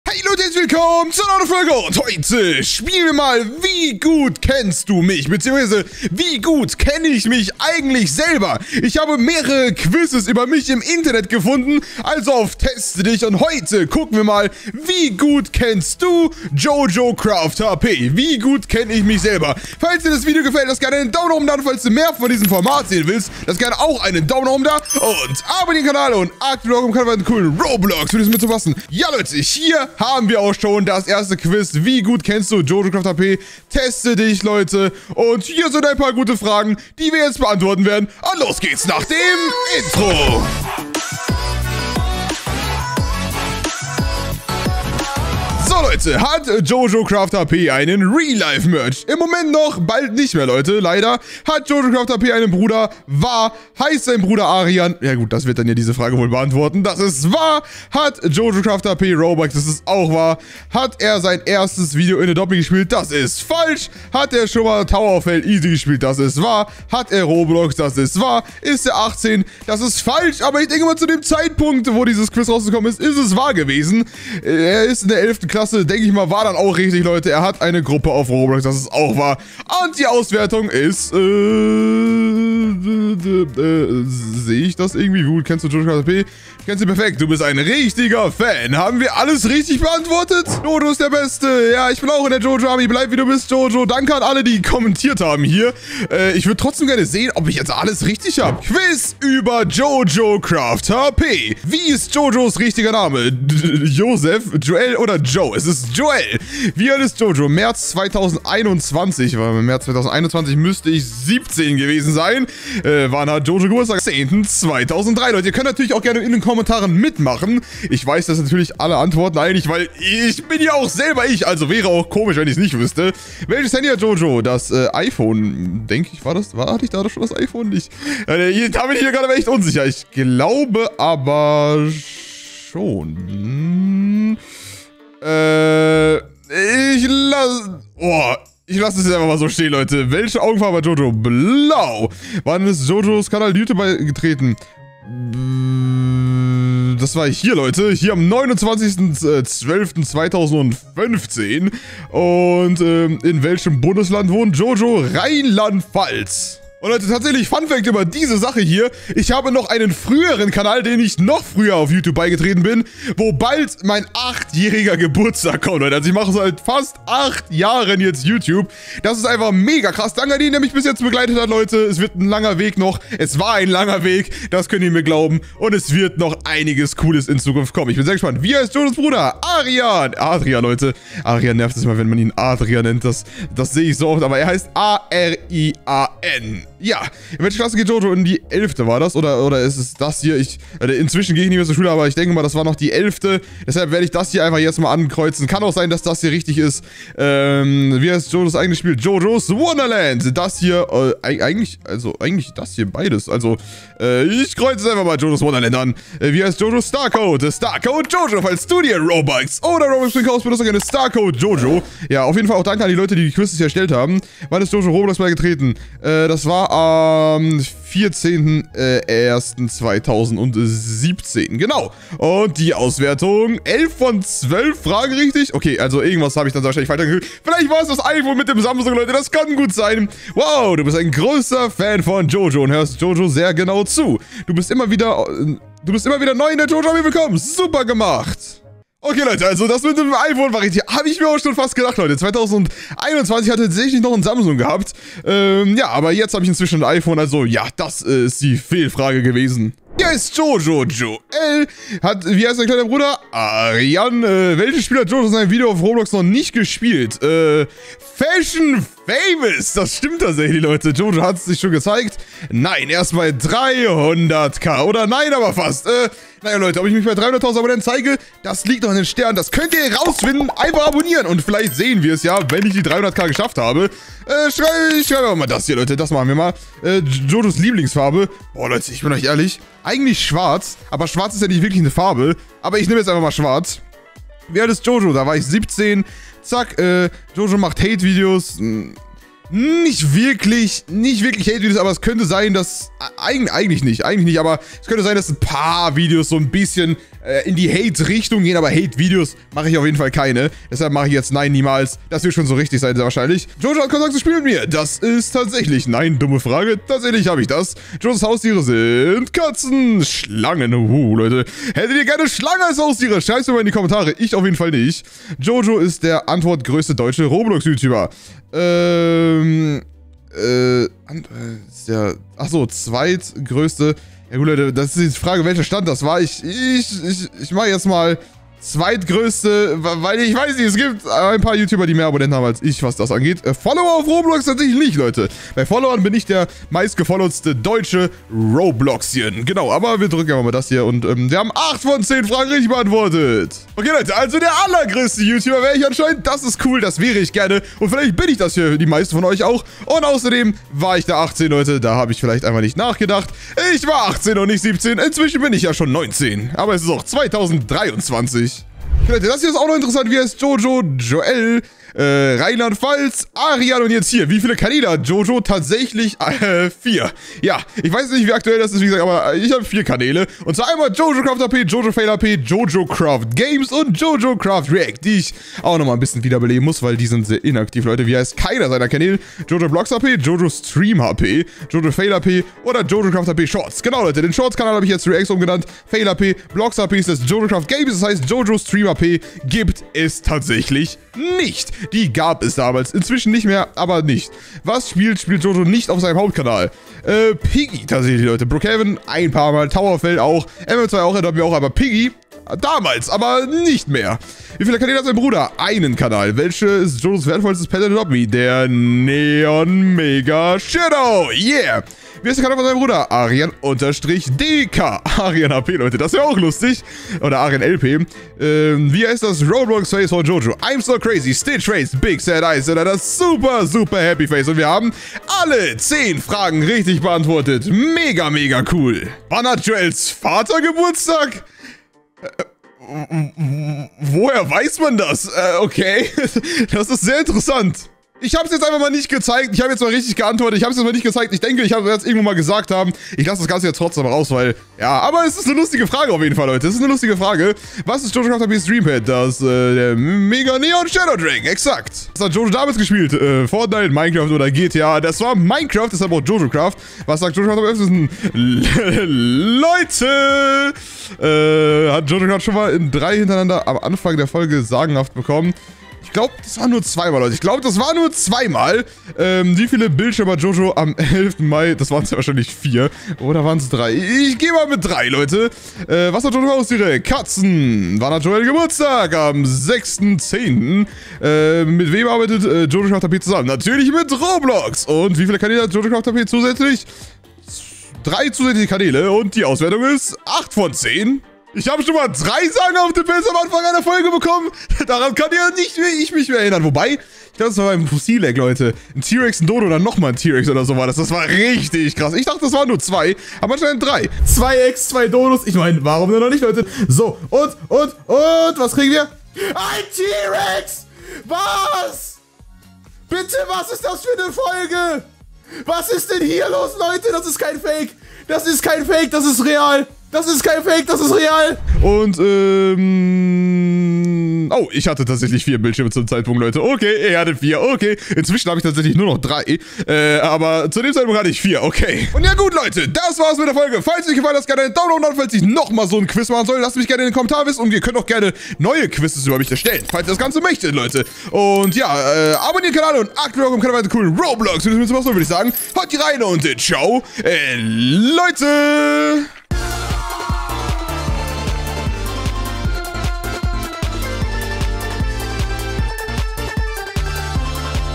Das JoJocraftHP QUIZ! Weiß ich alles über mich Selber? Teste dich
In diesem Video mache ich ein paar coole Teste dich von mir und schaue ob ich alles über mich selber weiß!
🌈Roblox Gruppe (Roblox Shop) ¹